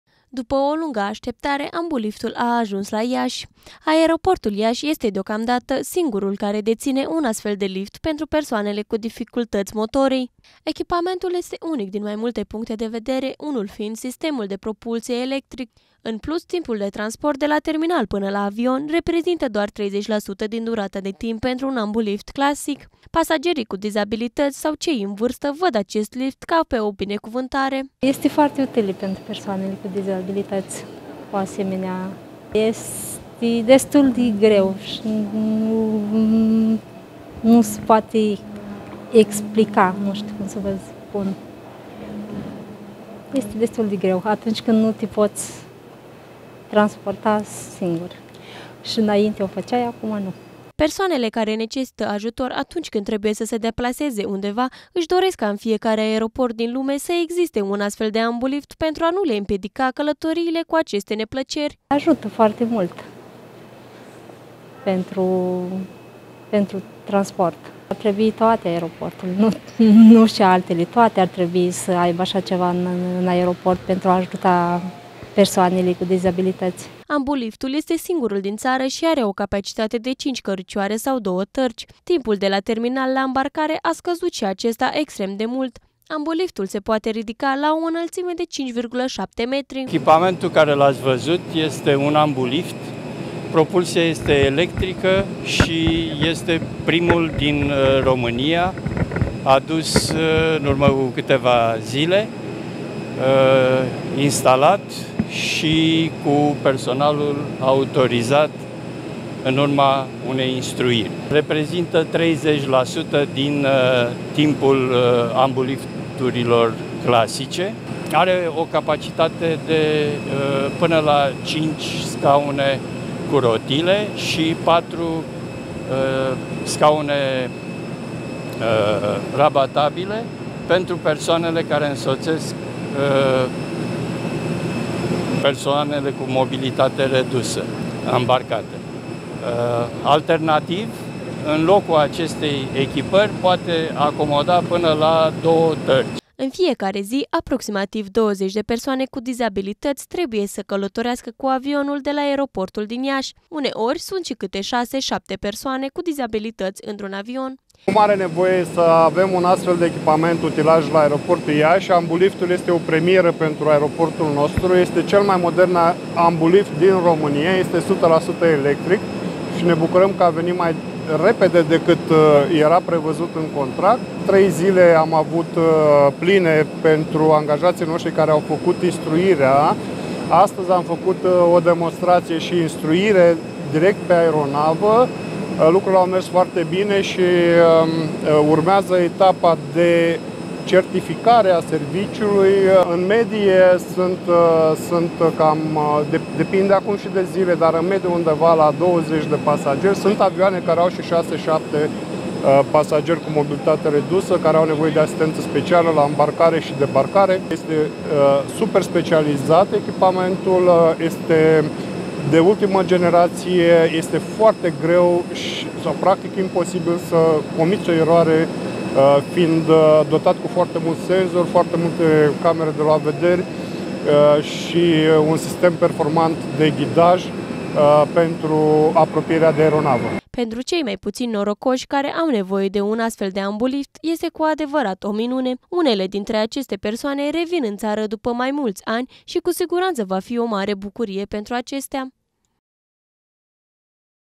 Singurul ambulift din țară este la Iași
Aeroportul Iași are, începând de joi, 8 august, un ambulift. Este primul echipament de acest fel din țară destinat persoanelor cu dificultăți motorii. Aceștia nu mai trebuie să își facă griji cum vor urca sau coborî din avion. Ambuliftul are o capacitate de cinci persoane în scaun cu rotile și patru scaune pentru însoțitori sau două tărgi.